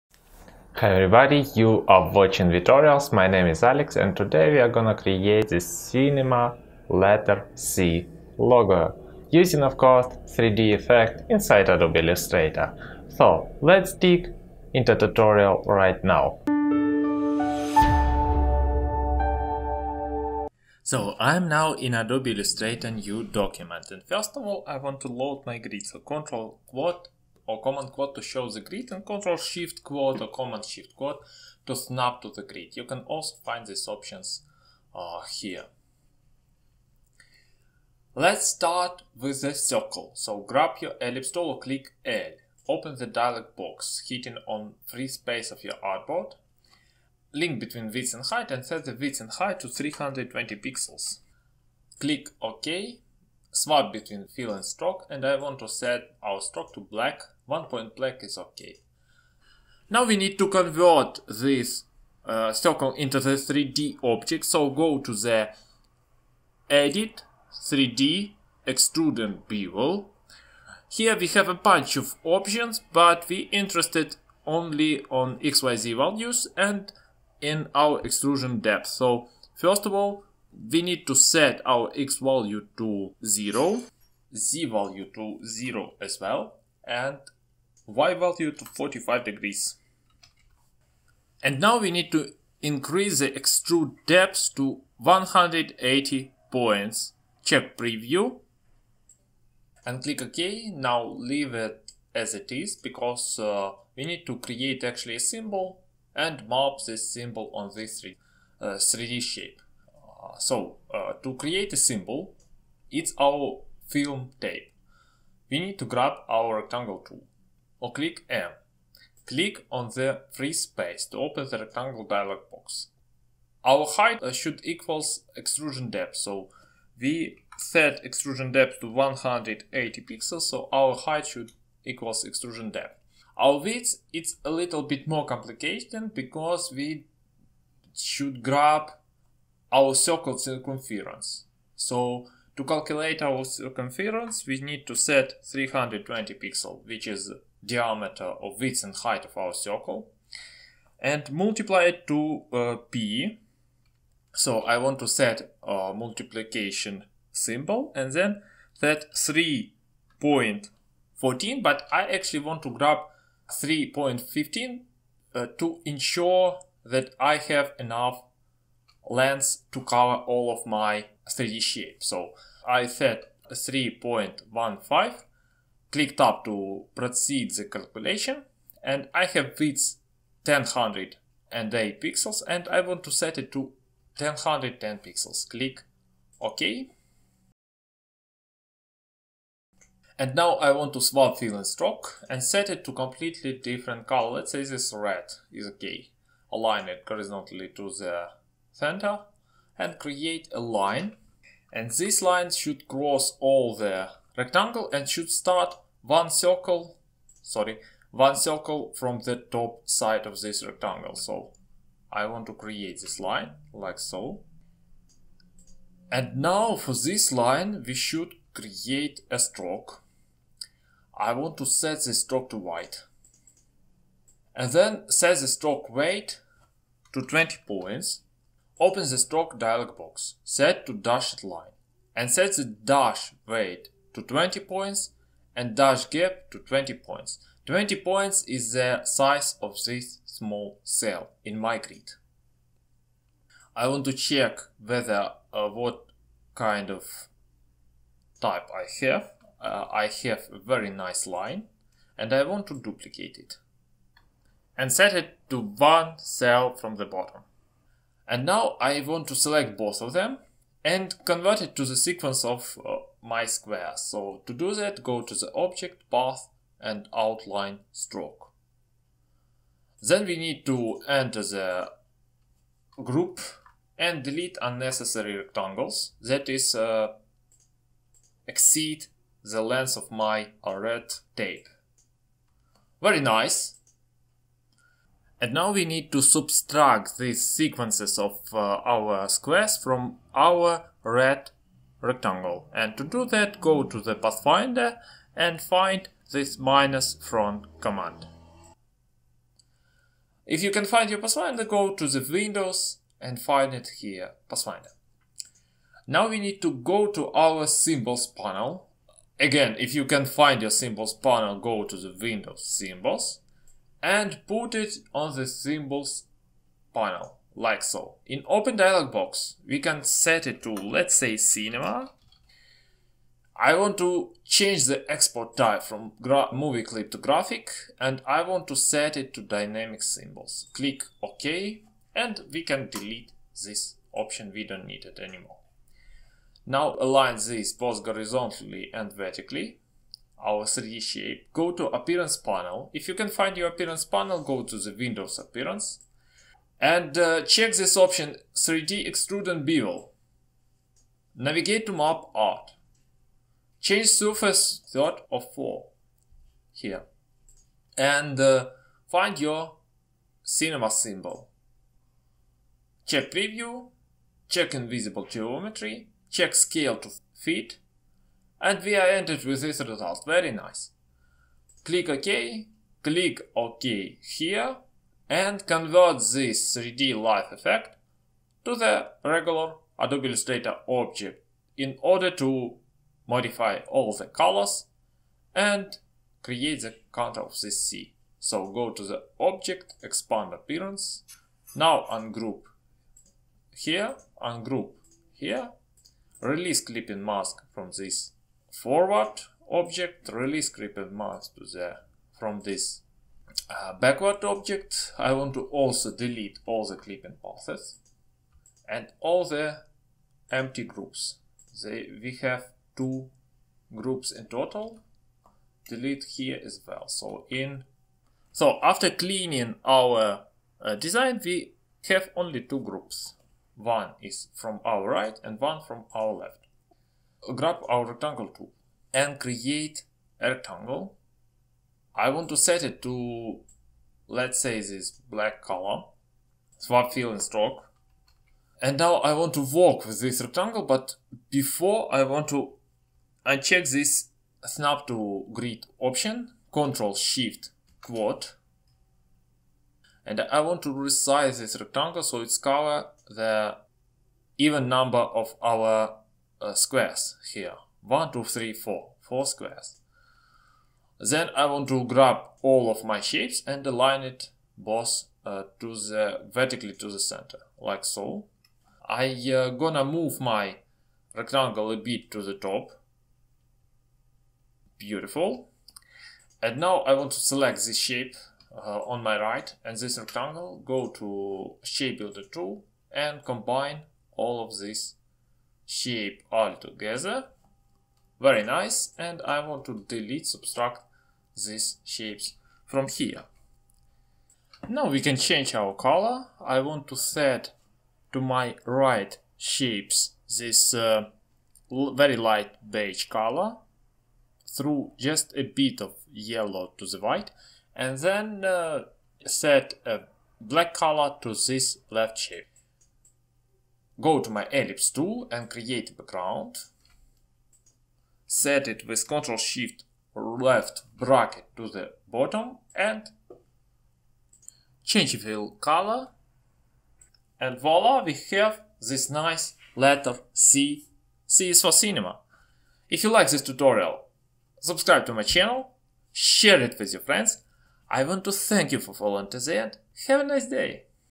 How to design letter C logo in Adobe Illustrator. Cinema tape logotype tutorial
Hi everybody. In today's tutorial, we are going to design a letter C logo in Adobe Illustrator.
In this particular case, we'll make 3d shape via Effect/3D/Extrude_and_bevel and then mapping symbol on its surface.
let' start!

1. Intro - 00:00
2. Create circle -
3. 3d object -
4. Symbol -
5. Letter C -